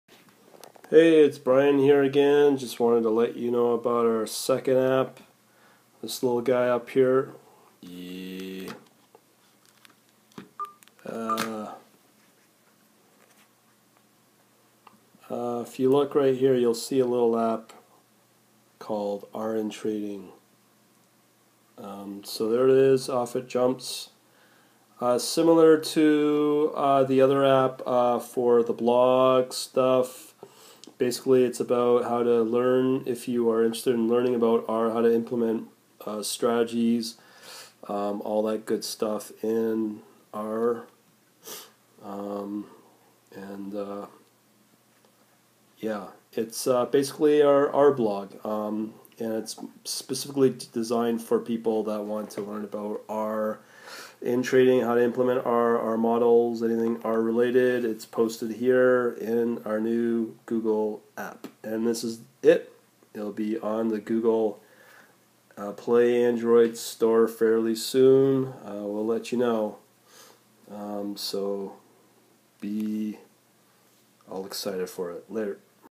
Learn how to do trading with R to build financial models, strategies,, HFT, forex on Google Android
This will be on the Google Play Android app store soon